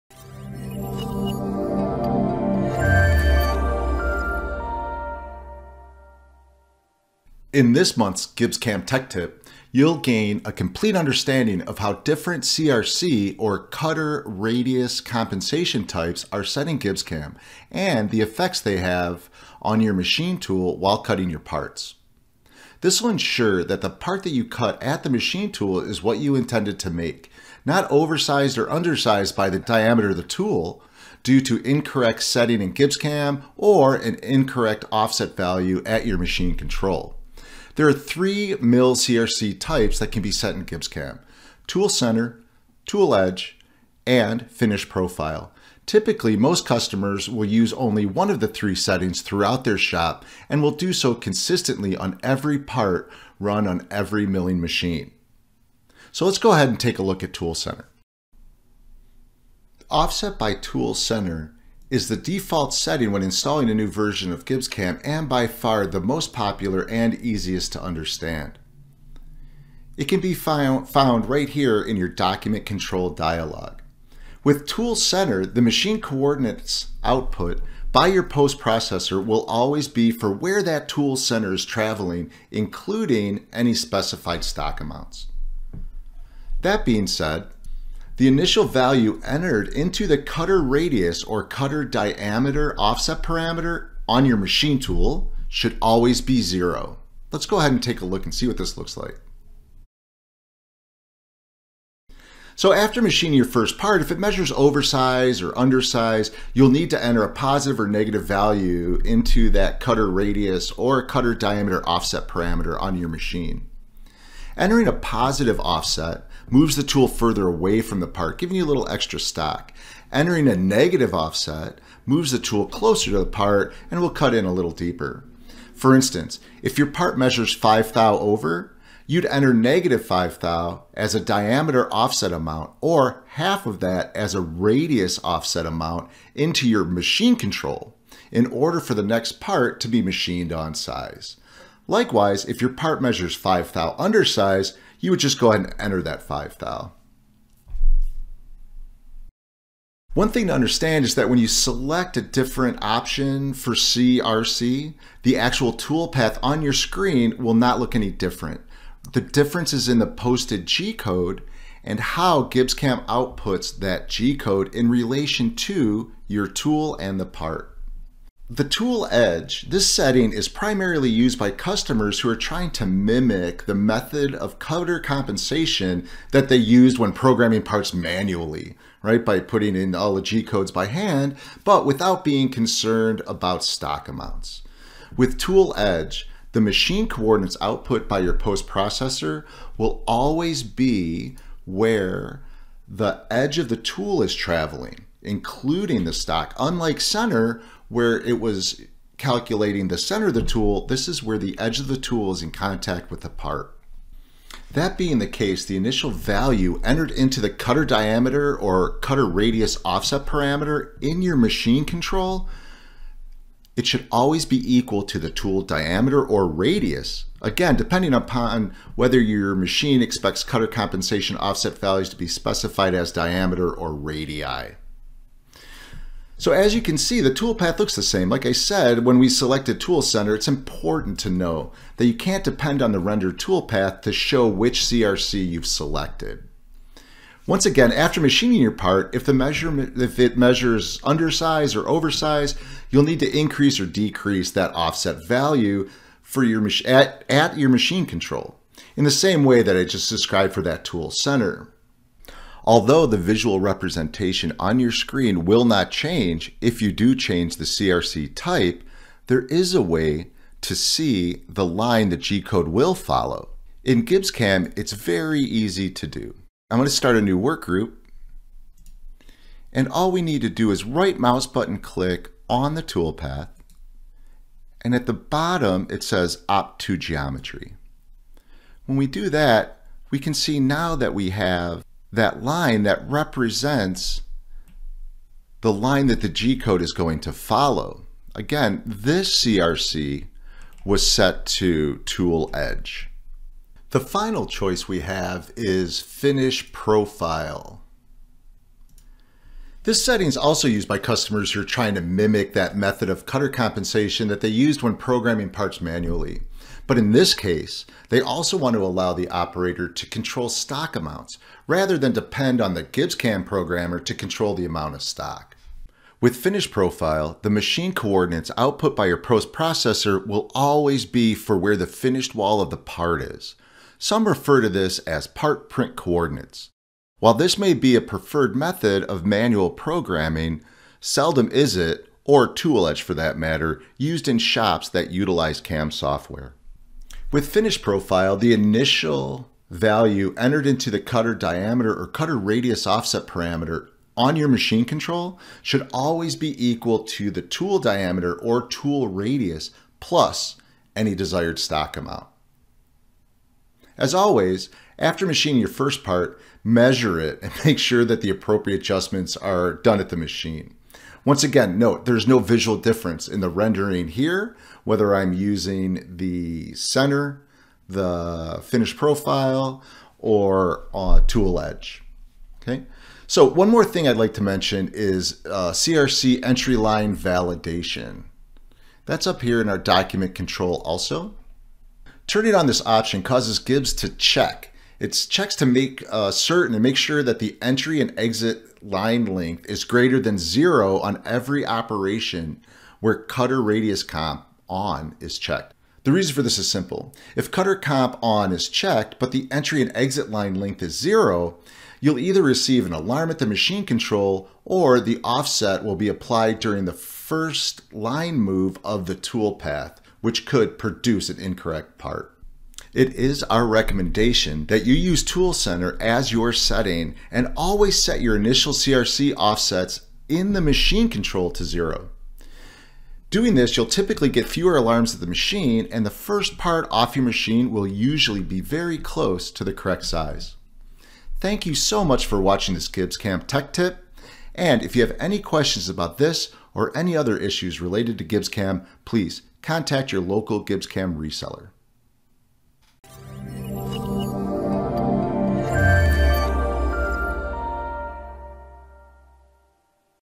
GibbsCAM Tech Tip: Understanding CRC Types
This GibbsCAM Tech Tip explains the Cutter Radius Compensation (CRC) types available to you in GibbsCAM.

There are multiple CRC types in GibbsCAM. It’s important to understand each one and the effect it will have on your toolpath and your machine settings. Incorrect settings in GibbsCAM or your machine control can give you unexpected results, including under or oversized parts.

Choosing the best setting for your shop is quick, easy, and ensures accurate parts every time!